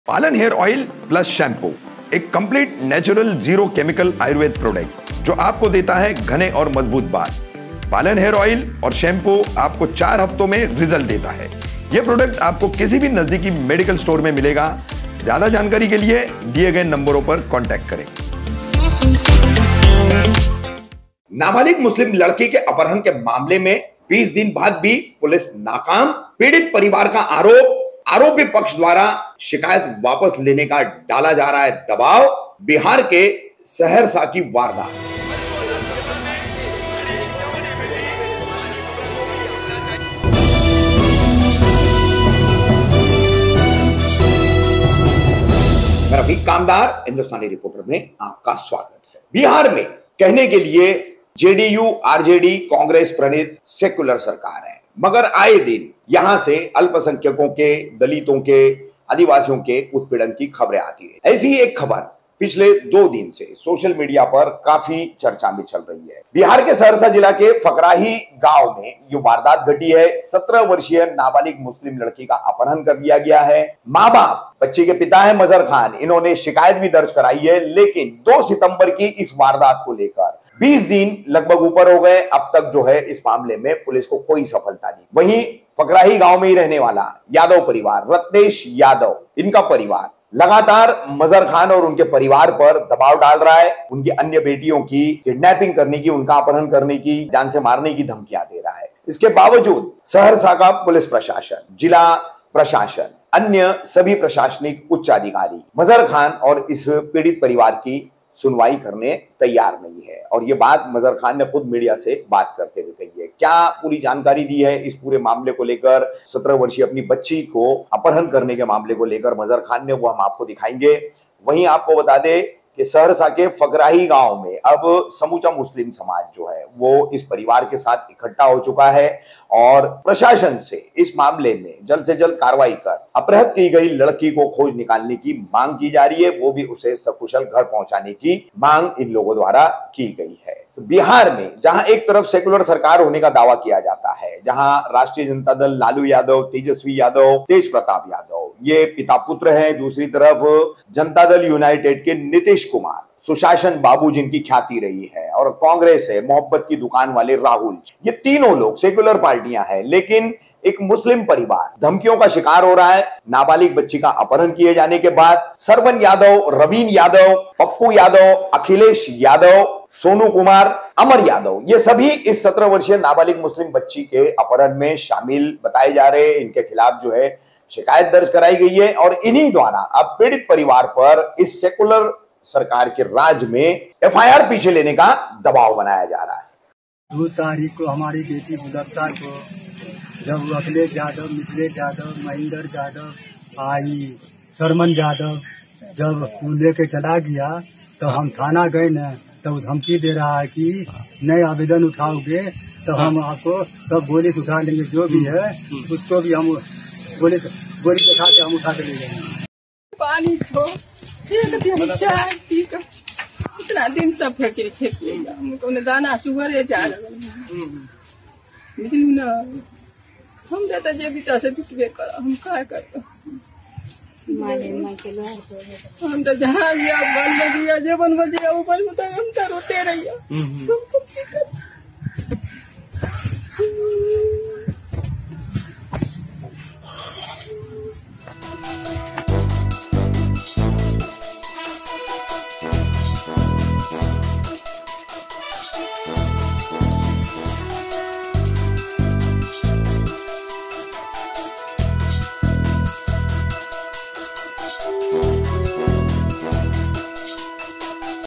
नाबालिग मुस्लिम लड़की के अपहरण के मामले में 20 दिन बाद भी पुलिस नाकाम | Hindustani Reporter
नाबालिग मुस्लिम लड़की के अपहरण के मामले में 20 दिन बाद भी पुलिस नाकाम

पीडित परिवार पर एफआईआर वापस लेने के लिए आरोपी डाल रहे हैं दबाव

बिहार के सहरसा जिले की वारदात